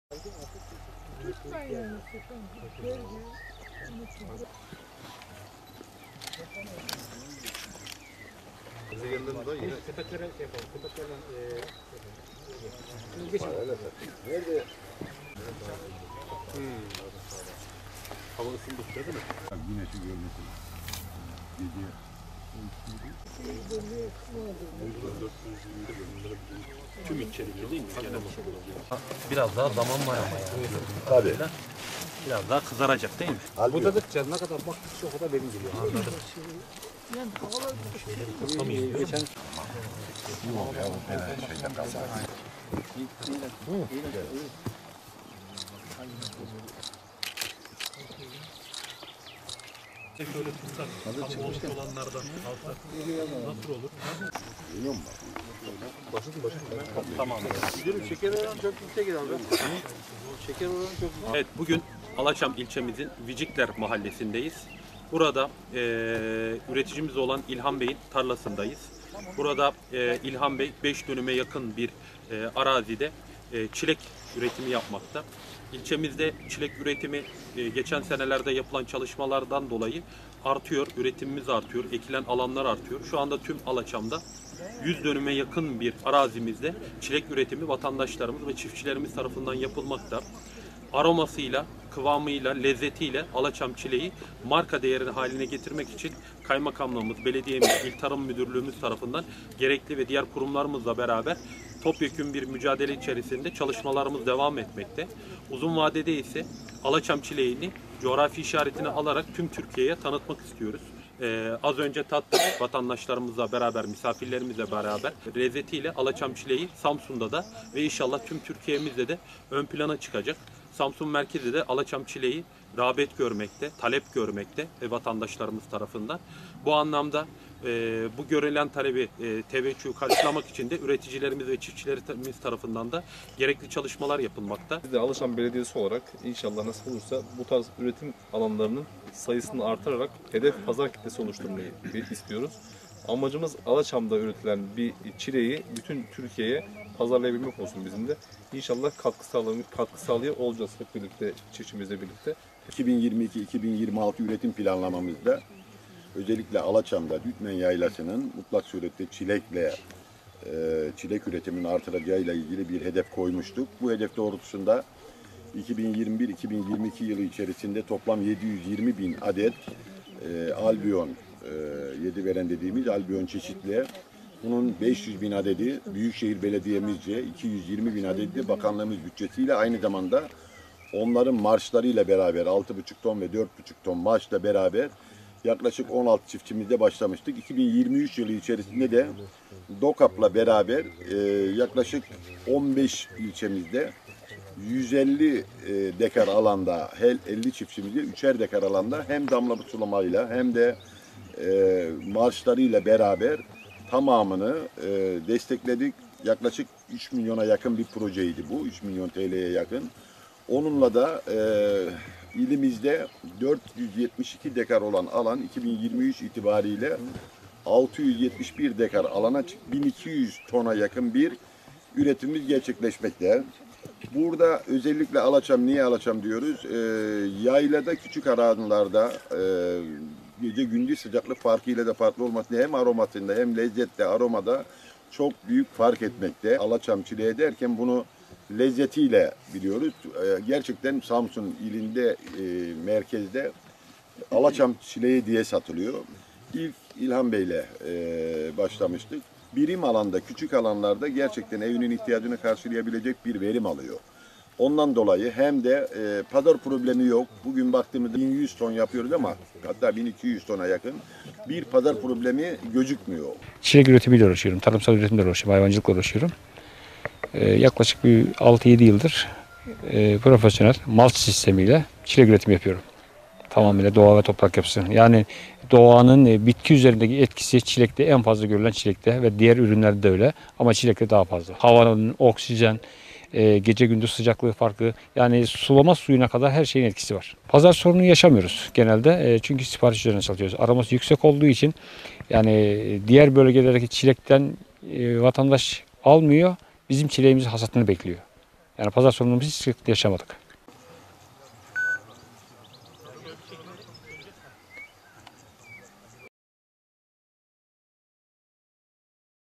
Bin rakımda yetişen çilek, pazar sorunu yaşamıyor
Samsun'un Alaçam ilçesi Dürtmen Yaylası'nda bin rakımda yetişen ve özel aroması ile damaklara hitap eden çilek, pazar sorunu yaşamıyor.